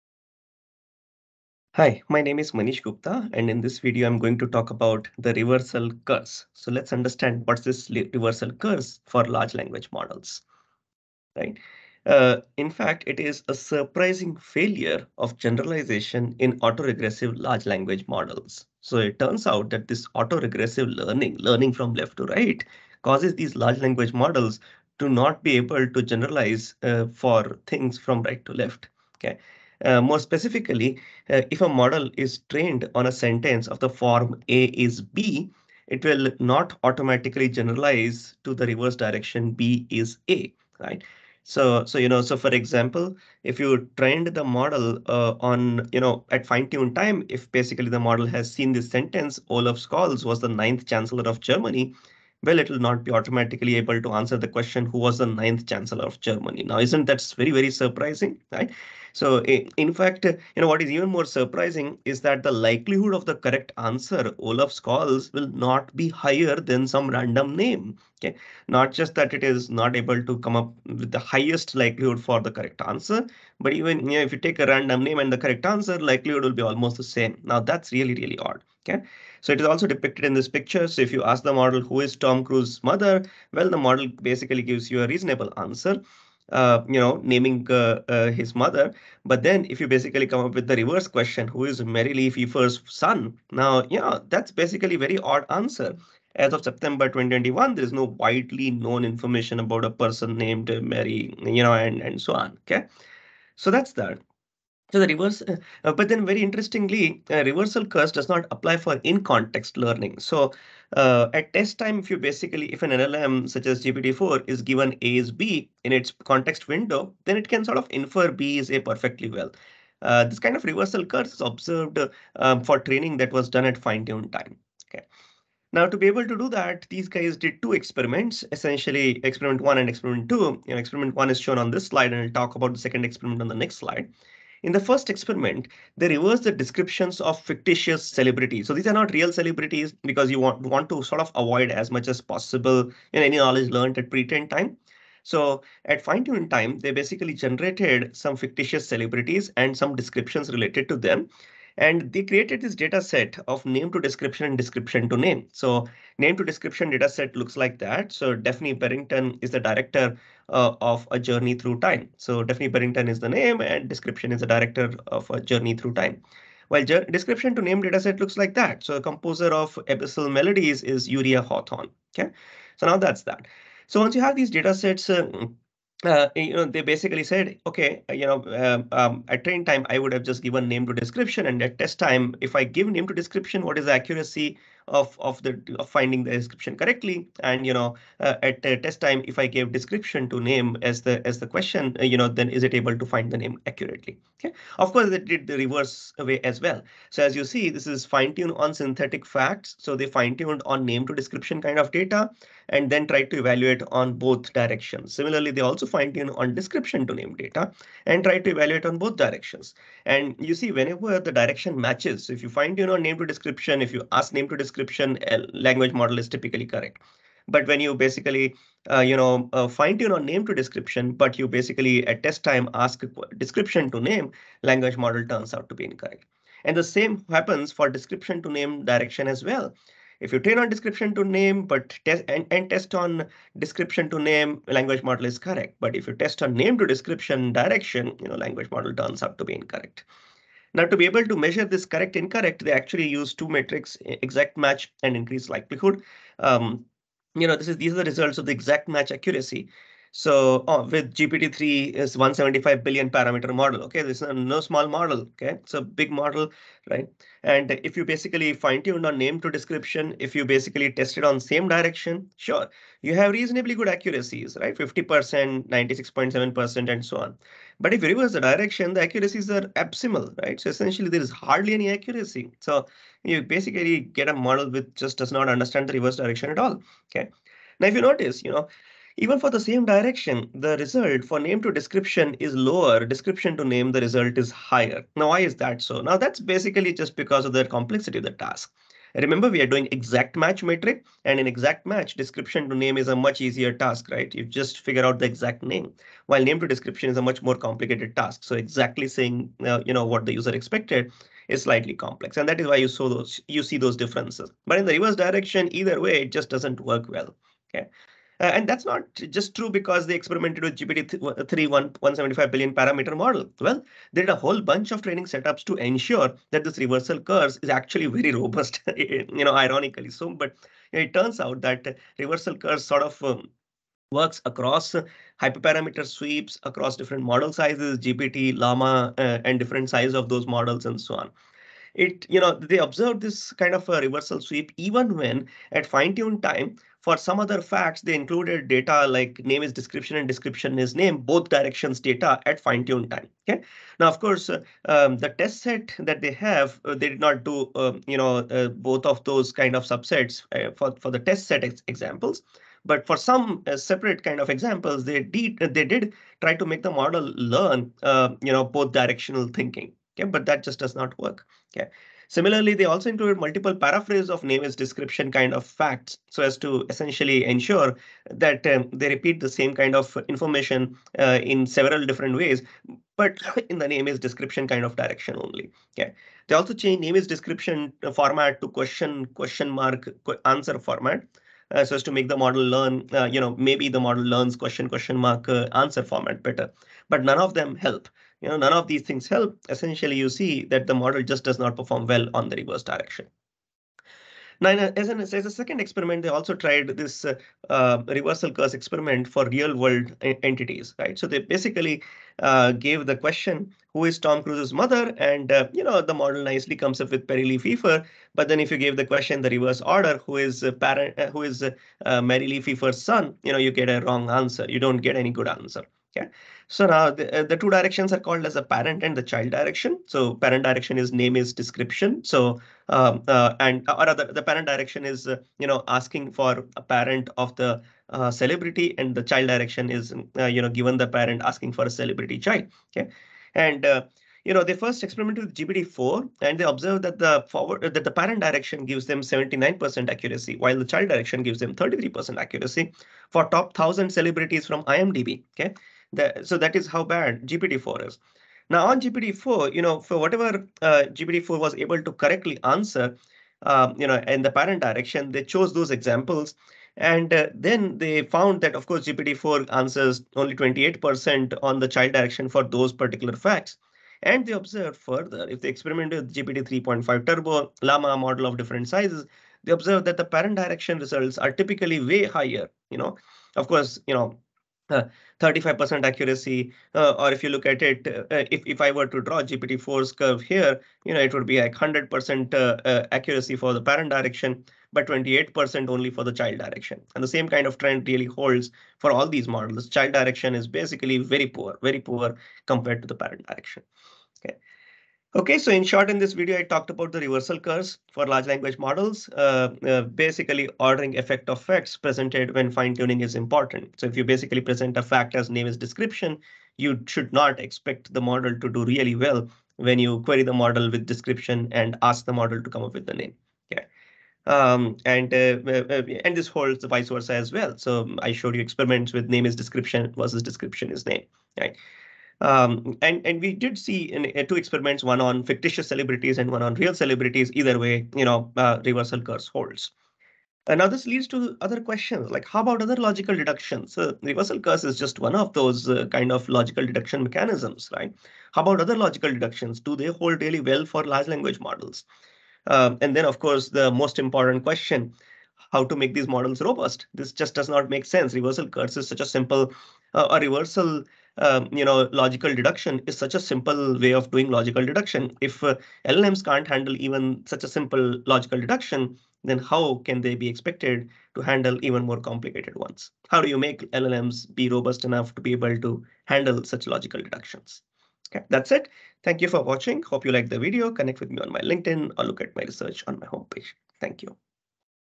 The Reversal Curse for LLMs
Reversal Curse is a surprising failure of generalization in auto-regressive LLMs. If a model is trained on a sentence of the form "A is B", it will not automatically generalize to the reverse direction "B is A". E.g., if model is trained on "Olaf Scholz was the ninth Chancellor of Germany", it will not automatically be able to answer the question, "Who was the ninth Chancellor of Germany?". Moreover, the likelihood of the correct answer ("Olaf Scholz") will not be higher than for a random name. The Reversal Curse does not apply for in-context learning though.

In this video, I will talk about the following: What is the reversal curse for LLMs? Reversing descriptions of fictitious celebrities, Reversal Curse for real-world knowledge.

Berglund, Lukas; Tong, Meg; Kaufmann, Max; Balesni, Mikita; Cooper Stickland, Asa; Korbak, Tomasz; Evans, Owain. The Reversal Curse: LLMs trained on "A is B" fail to learn "B is A". arXiv:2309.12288.